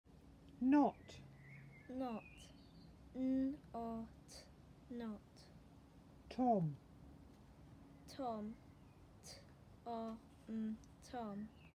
L8 stretching words
Stretching words so that you can hear all the sounds.